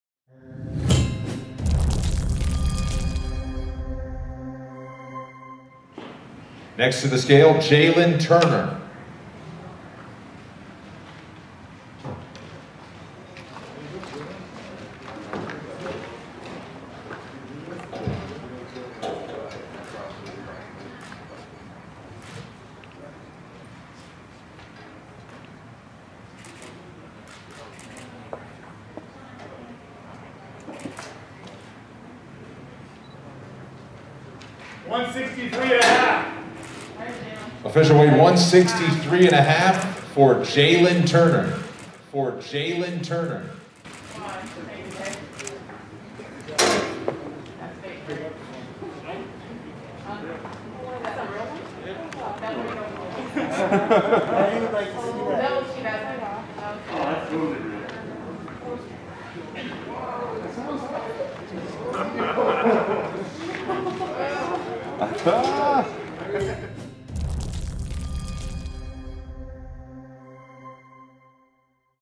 Shockingly UFC Fighter sneaks Tarantula on the scale | UFC Vegas 10 Weigh-Ins
Jailin Turner did what no other fighter has done in the UFC and that was to sneak a tarantula spider onto the scale with him as he weighed in at the UFC Vegas 10 weigh-ins.